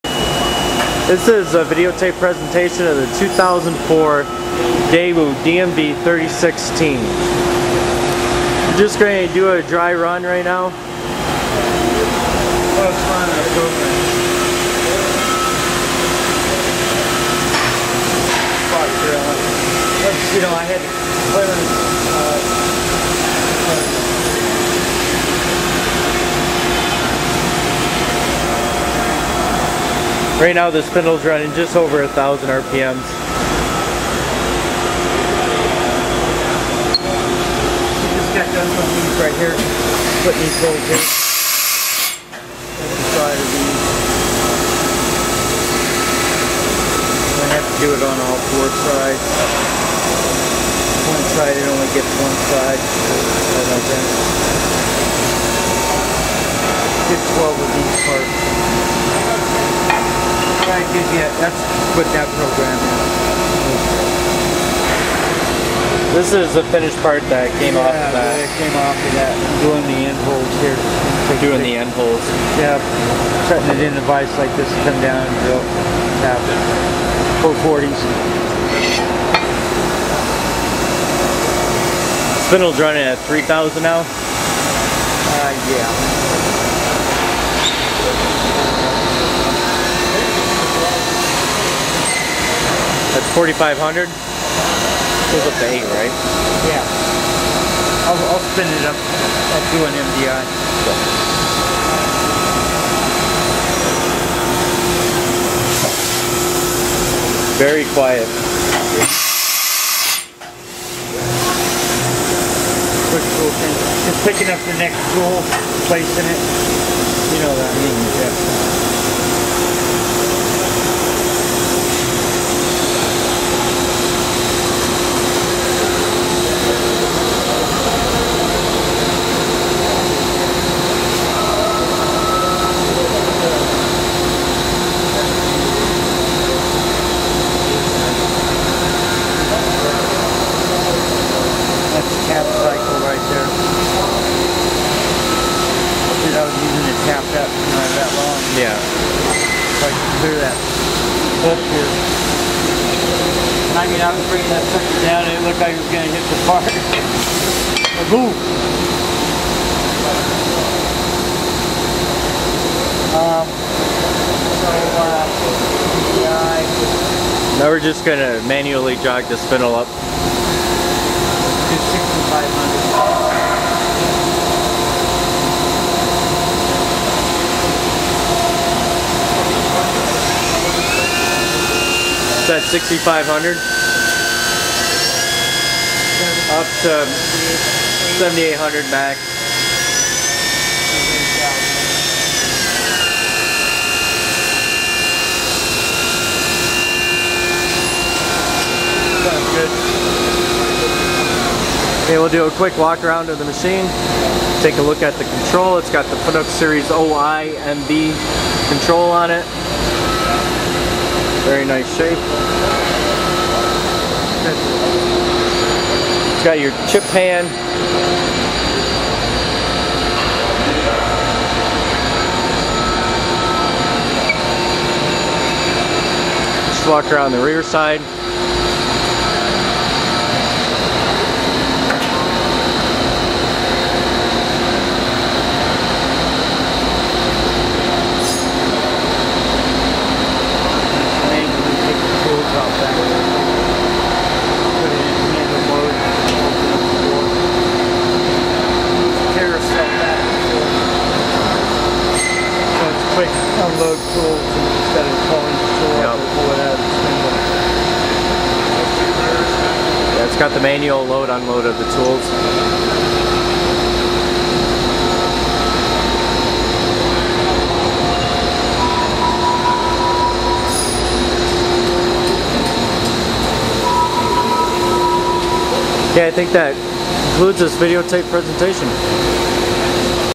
321160 2005 Daewoo DMV 3016 Vertical Machining Center
Specifications:
    Table size:                             47" x 20"
    Max. table load:                        1,320 lbs
    Spindle nose to table top:              6" - 26"
    Rapid traverse rate (X/Y):              1,181 ipm
    Rapid traverse rate (Z):                945 ipm
    Spindle taper:                          CAT40
    Tool changer capacity:                  30 atc side mount
    Dimensions (approx.):                   116"L x 99"W x 108"H

Equipped with (must be verified by buyer)
    Fanuc Oi-MB CNC control